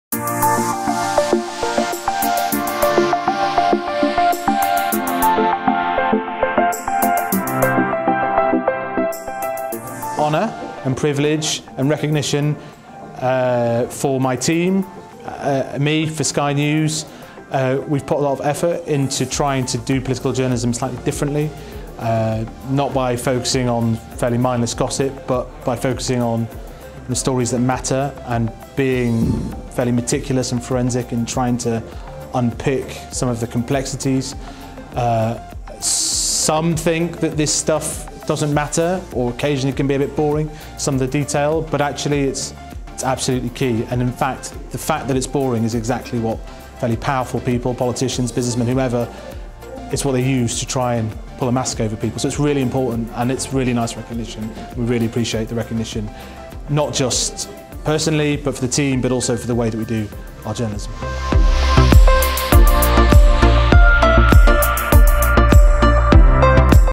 Faisal Islam - Journalist of the Year - Asian Media Awards 2018
Winner of the Journalist of the Year Award 2018 supported by Manchester Metropolitan University & Routes Into Languages North West - Faisal Islam, Sky News.

Award was presented by Senior Lecturer in Multimedia Journalism and Programme Leader MA Multimedia Journalism at the Manchester Metropolitan University Andrew Dickinson.

Interviews conducted by University of Salford students. The student team was led by Grace Mailey & Ioan Bustean.